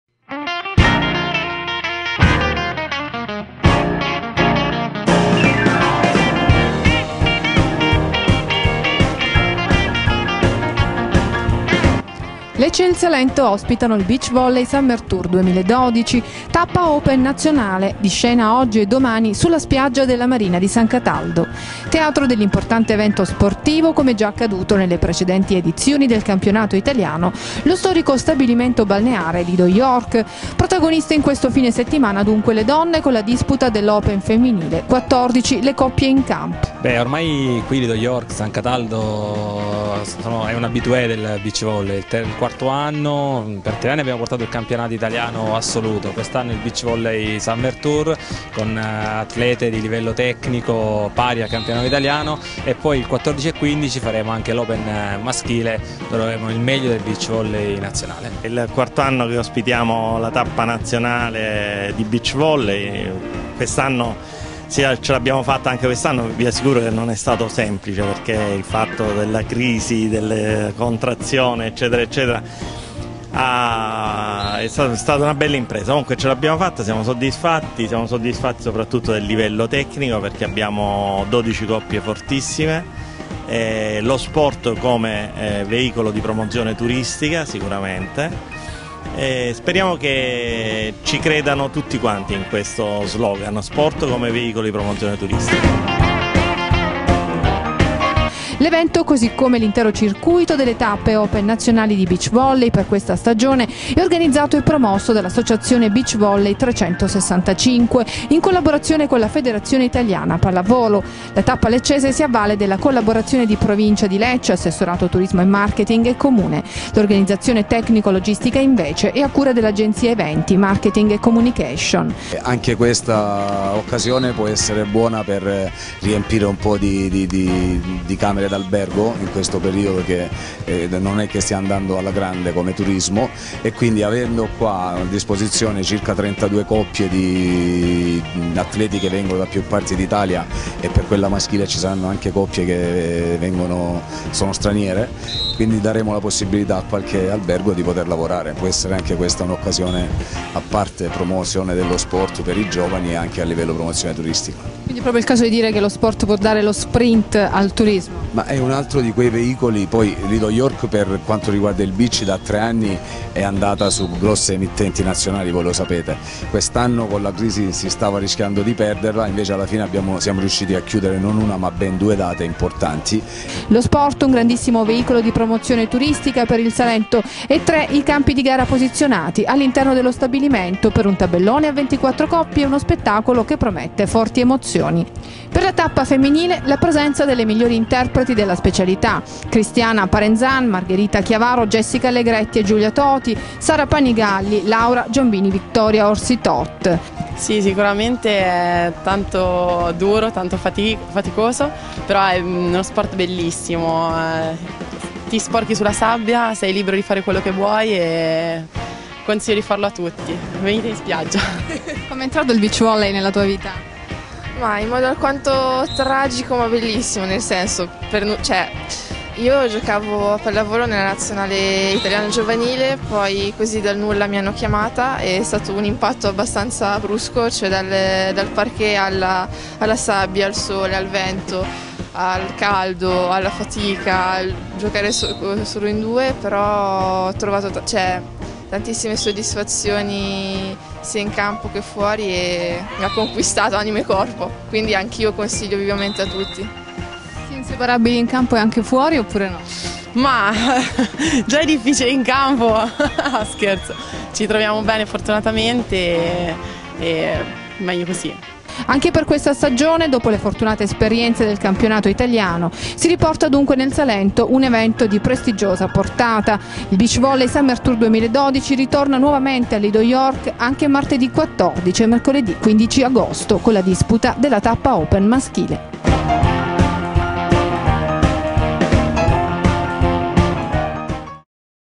"BEACH volley summer tour 2012", oggi e domani a San Cataldo
Al via oggi il "Beach Volley Summer Tour 2012", tappa Open nazionale, di scena fino a domani (domenica 5 agosto) sulla spiaggia della marina di San Cataldo, nello storico stabilimento balneaare "Lido York". Protagoniste le donne, con la disputa dell'Open femminile (14 le coppie in campo).
L'evento, così come l'intero circuito delle tappe open nazionali di beach volley per questa stagione, è organizzato e promosso dall'Associazione BeachVolley 365 in collaborazione con la Federazione Italiana Pallavolo. La tappa leccese si avvale della collaborazione di Provincia di Lecce (assessorato Turismo e Marketing territoriale) e Comune di Lecce. L'organizzazione tecnico-logistica è a cura dell'agenzia Eventi Marketing & Communication.